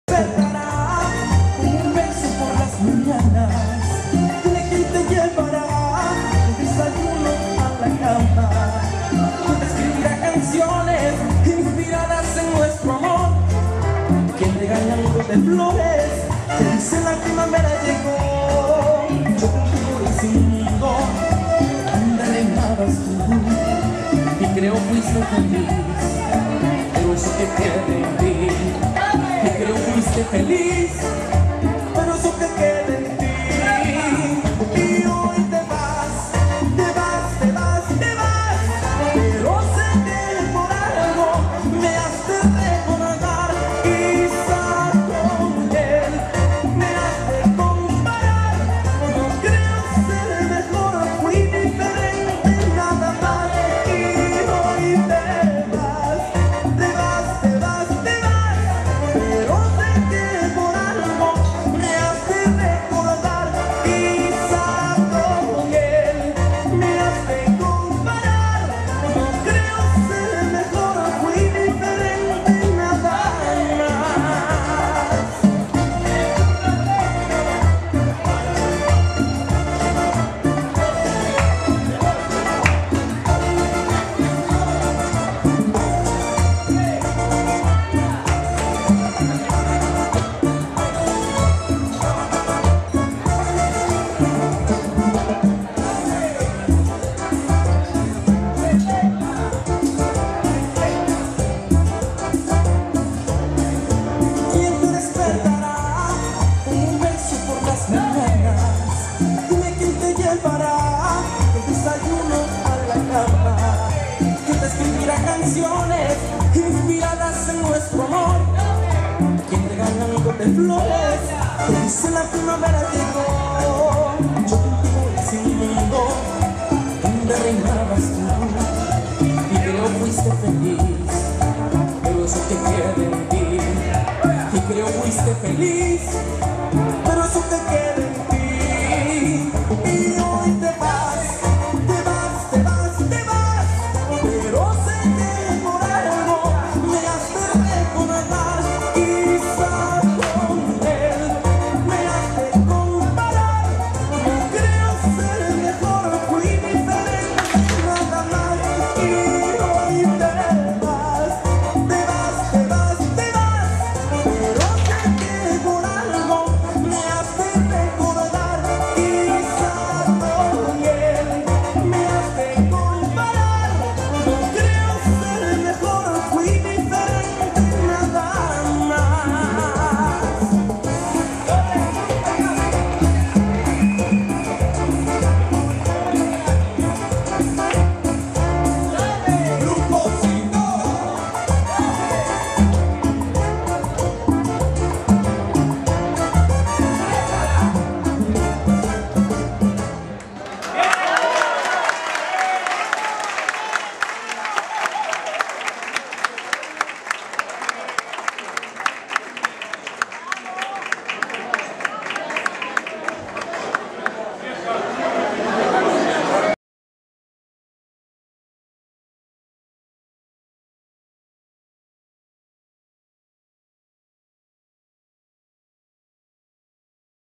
Rueda Cubana 2012.
Exhibicion Rueda Cubana Vigosalsa. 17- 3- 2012
Redondela.  Cena Benéfica a favor de la Asociación contra el cancer. Salones Monasterio de Redondela